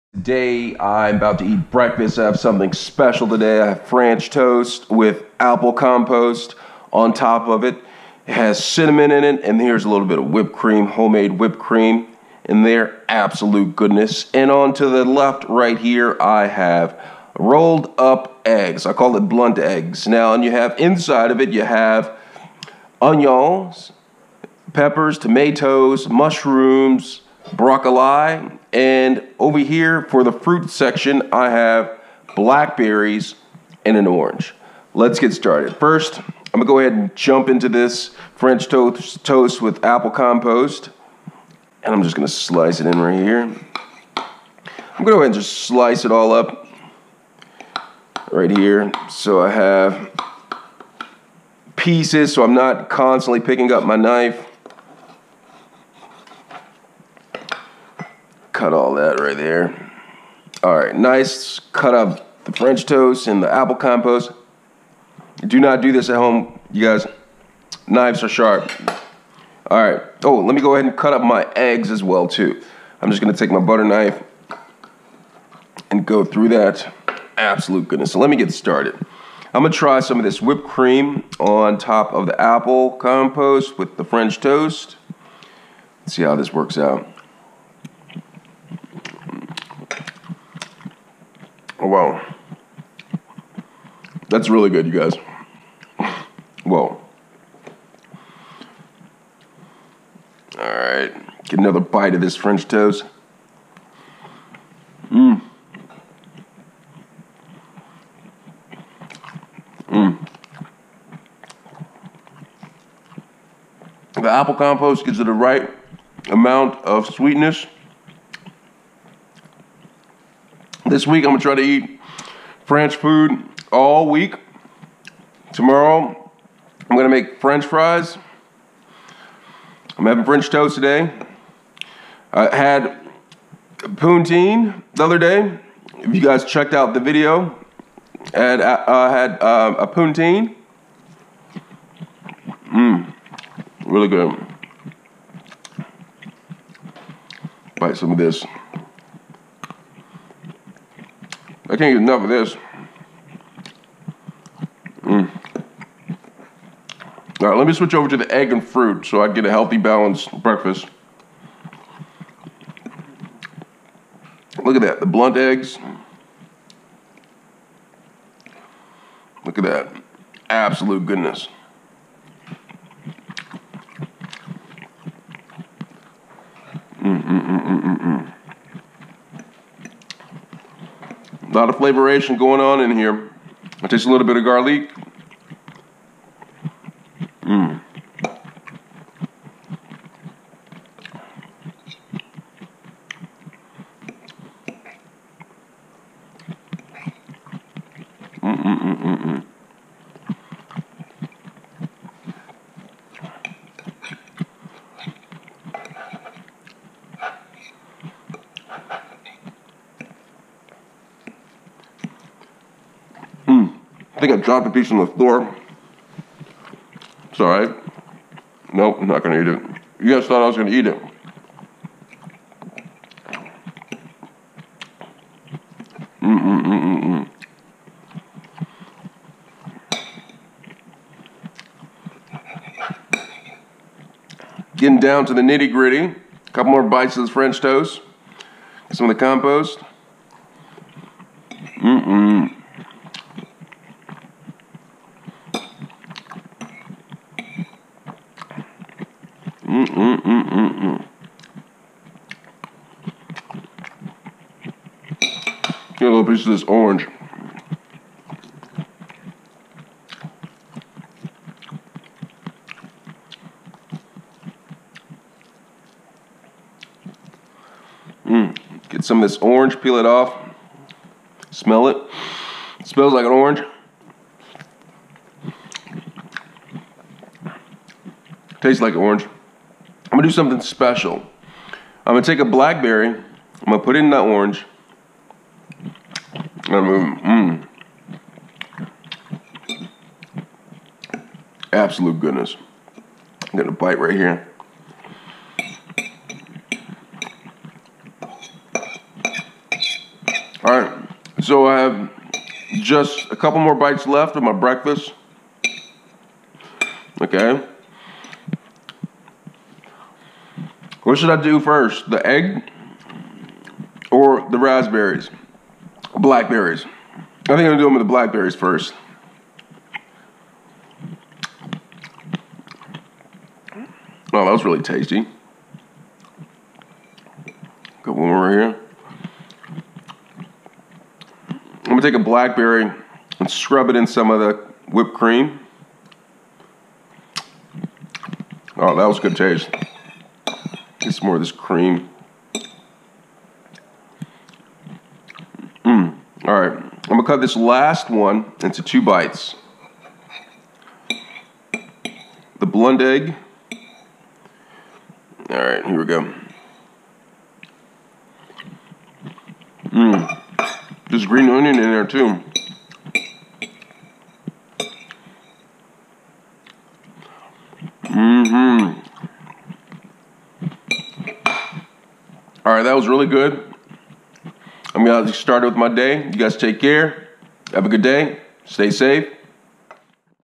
Franch Toast and Blunty-Rolled Eggs with Fruit (Mukbang)
Eating, Home Franch Toast and Blunty-Rolled Eggs with Fruit. Home  (Mukbang) .